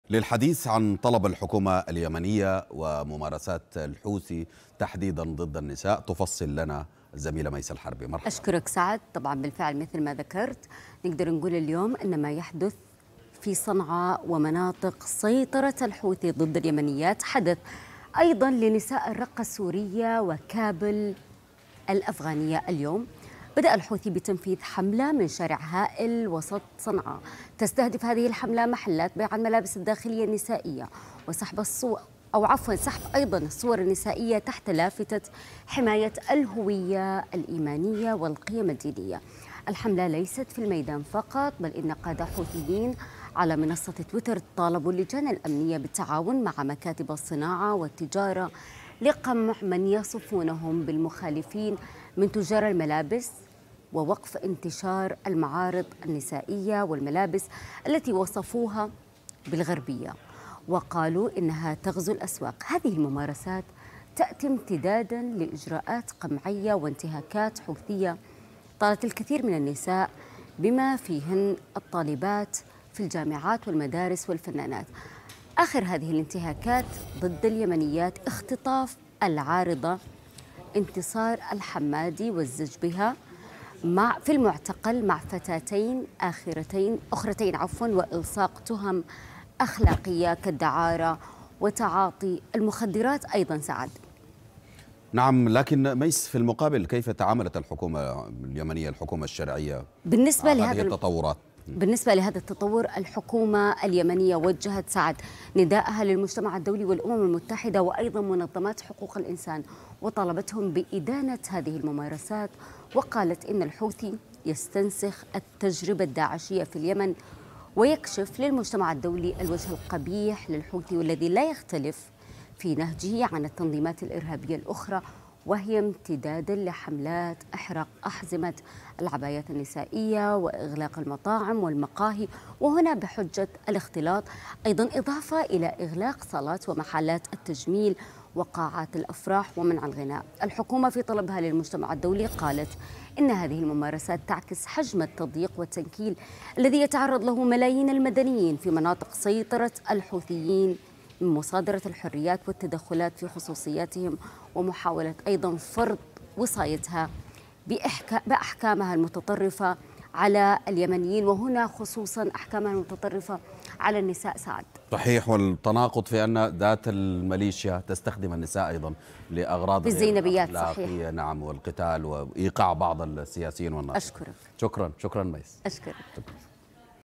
بأساليب "داعش" والقاعدة.. الحوثي يقمع النساء اليمنيات
ممارسات قمعية تنفذها ميليشيات_الحوثي على النساء اليمنيات.. مسلسل مشابه لجرائم داعش في الرقة السورية و القاعدة في كابول الأفغانية